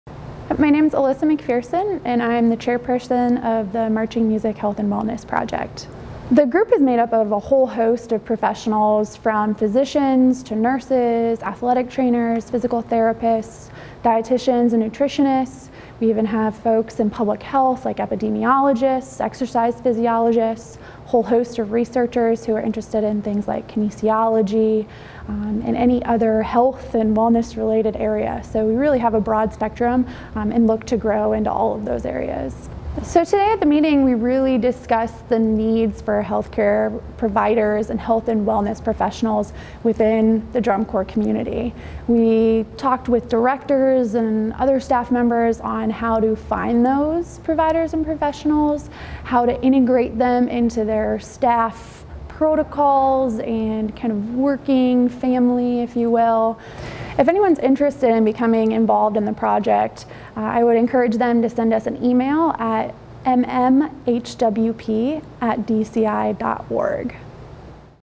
Marching Music Health & Wellness Project
Alyssa McPherson, chairperson of the Marching Music Health & Wellness project, presented to corps directors during the 2017 DCI Annual Meeting in Atlanta on Saturday, January 28.